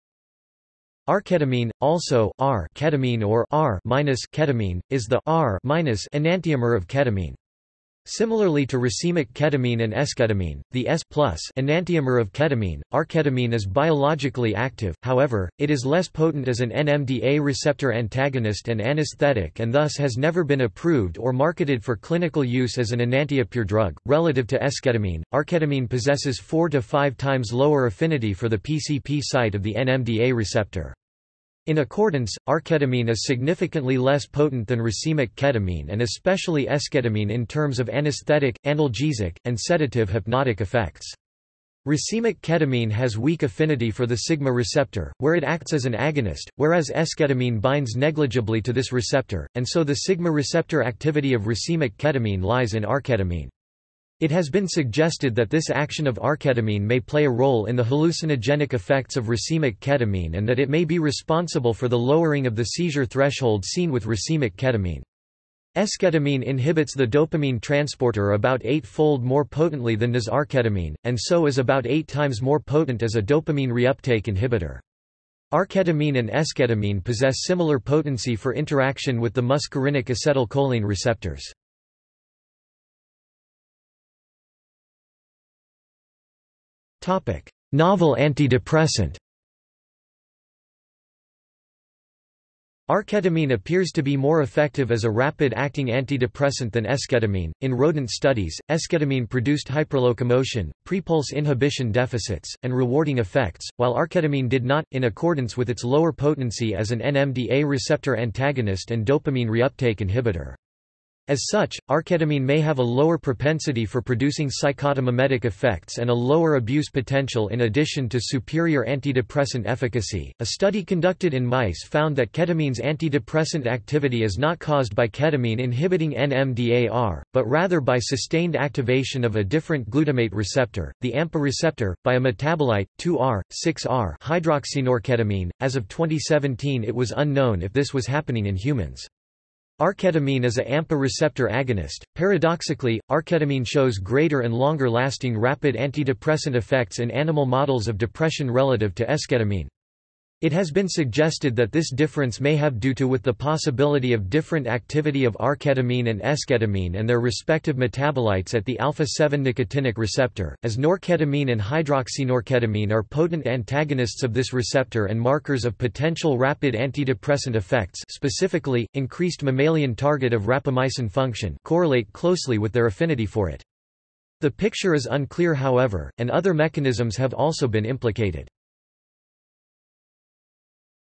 Arketamine | Wikipedia audio article
This is an audio version of the Wikipedia Article:
Arketamine

00:01:23 undefined

- Socrates

SUMMARY
Arketamine, also (R)-ketamine or (R)-(−)-ketamine, is the (R)-(−) enantiomer of ketamine. Similarly to racemic ketamine and esketamine, the S(+) enantiomer of ketamine, arketamine is biologically active; however, it is less potent as an NMDA receptor antagonist and anesthetic and thus has never been approved or marketed for clinical use as an enantiopure drug.Relative to esketamine, arketamine possesses 4–5 times lower affinity for the PCP site of the NMDA receptor. In accordance, arketamine is significantly less potent than racemic ketamine and especially esketamine in terms of anesthetic, analgesic, and sedative-hypnotic effects. Racemic ketamine has weak affinity for the sigma receptor, where it acts as an agonist, whereas esketamine binds negligibly to this receptor, and so the sigma receptor activity of racemic ketamine lies in arketamine. It has been suggested that this action of arketamine may play a role in the hallucinogenic effects of racemic ketamine and that it may be responsible for the lowering of the seizure threshold seen with racemic ketamine. Esketamine inhibits the dopamine transporter about 8-fold more potently than does arketamine, and so is about 8 times more potent as a dopamine reuptake inhibitor. Arketamine and esketamine possess similar potency for interaction with the muscarinic acetylcholine receptors.